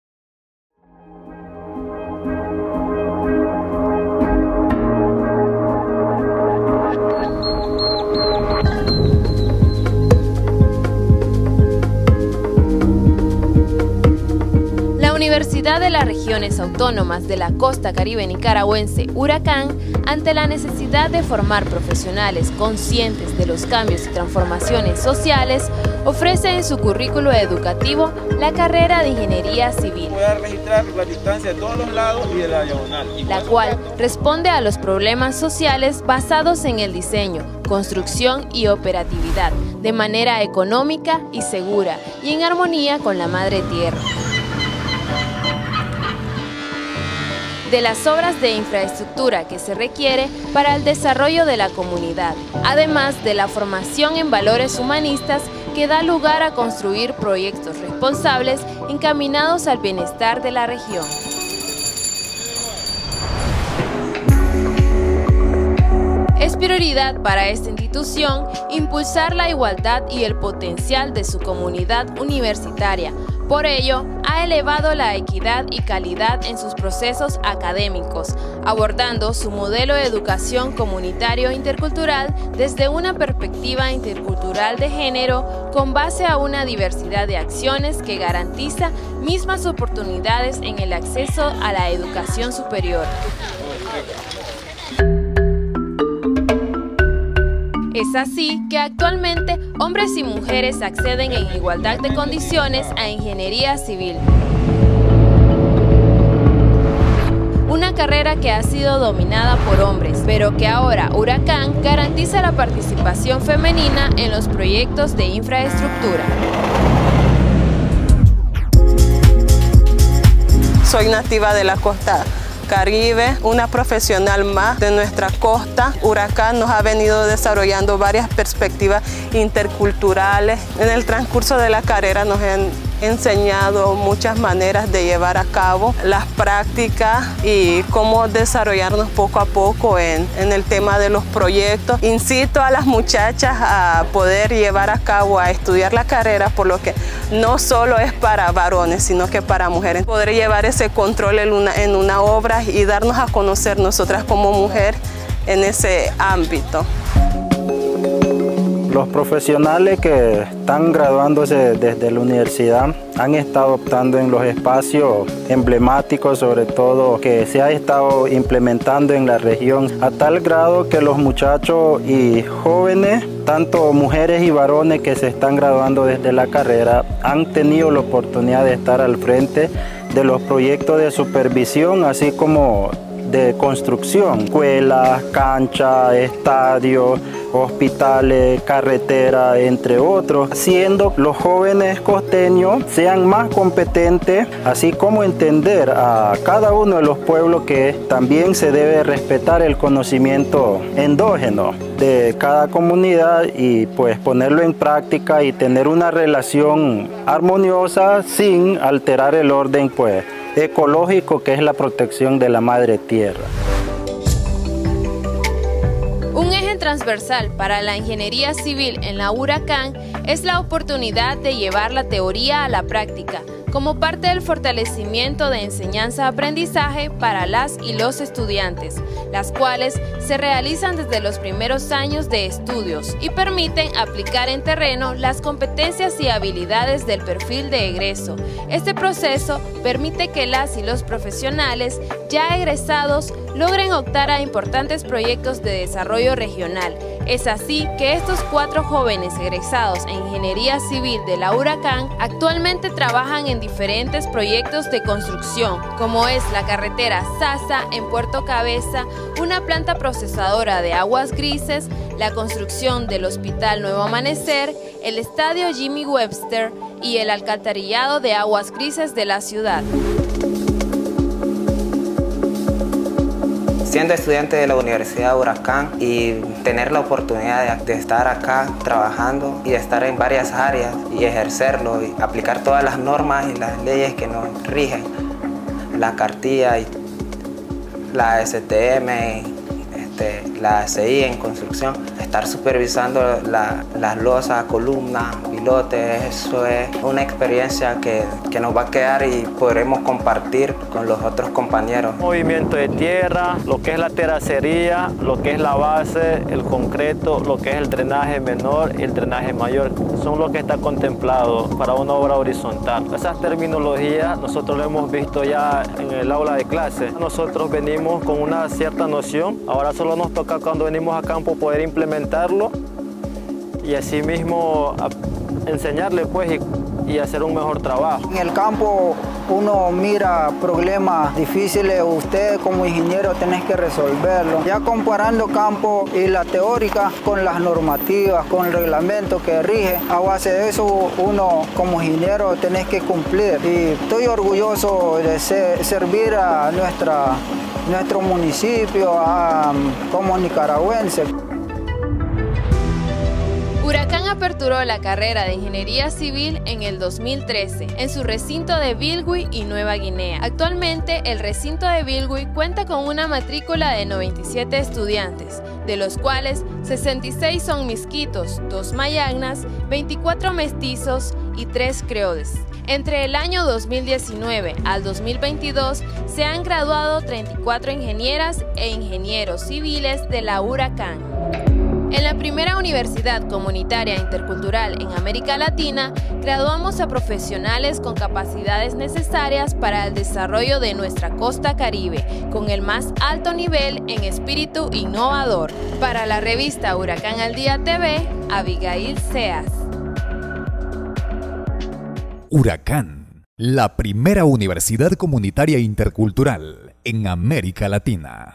𝐑𝐄𝐏𝐎𝐑𝐓𝐀𝐉𝐄 I 4 jóvenes graduados en Ingeniería Civil competentes en el aporte al desarrollo regional
URACCAN aperturó la carrera de Ingenieria Civil en el año 2013, en su recinto de Bilwi y Nueva Guinea, Un eje transversal, para la Ingenieria Civil en la URACCAN, es la oportunidad de llevar la teoría a la práctica, como parte del fortalecimiento de enseñanza aprendizaje para las y los estudiantes, las cuales se realizan desde los primero años de estudios y permiten aplicar en terreno las competencias y habilidades del perfil de egreso.